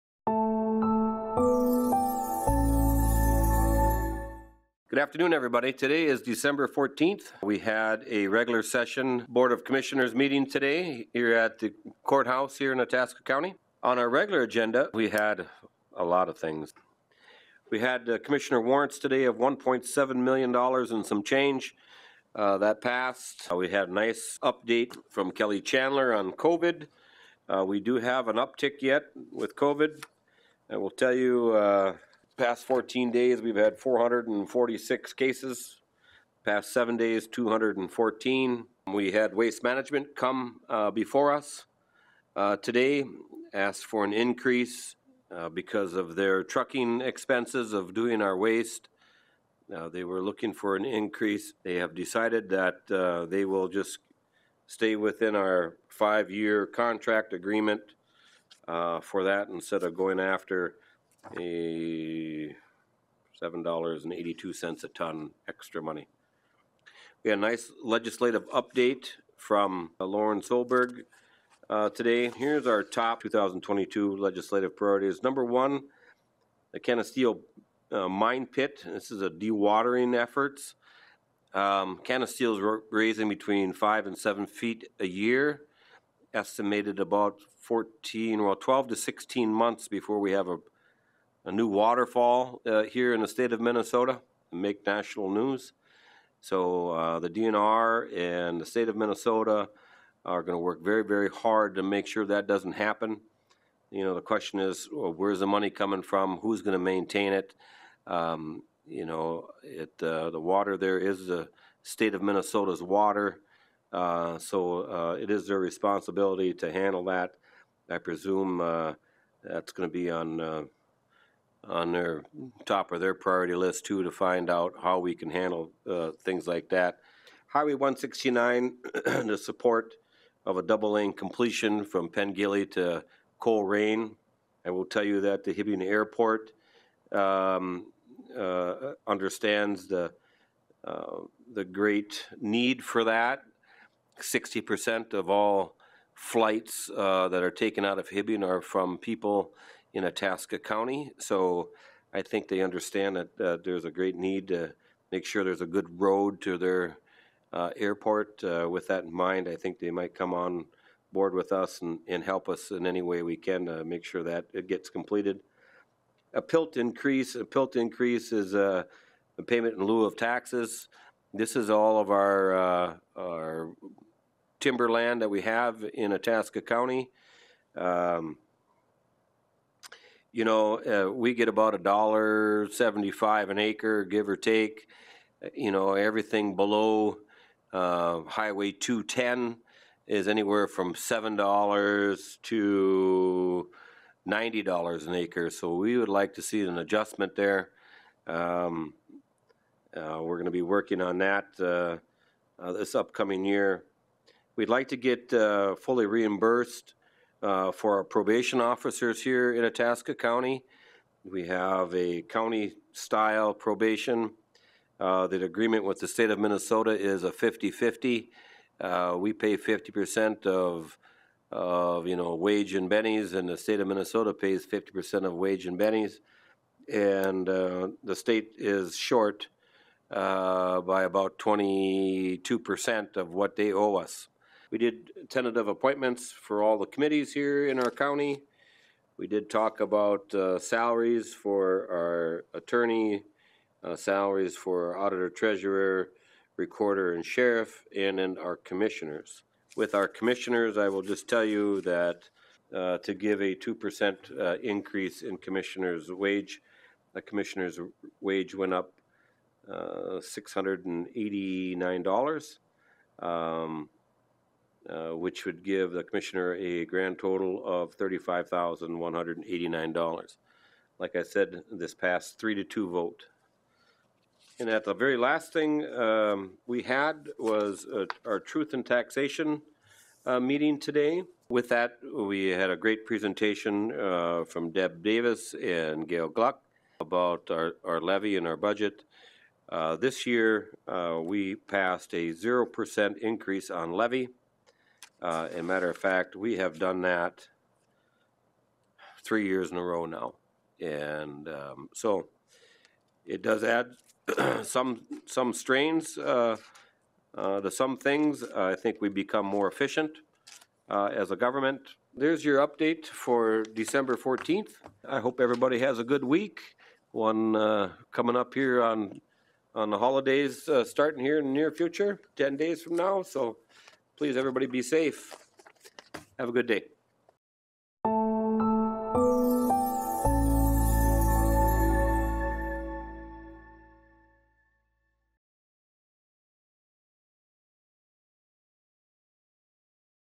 Community Matters, Burl Ives 12-15-2021
By a three to two vote the Itasca County Board of Commissioners approved an increase in their salaries of $689 for a grand total of $35,189 for 2022.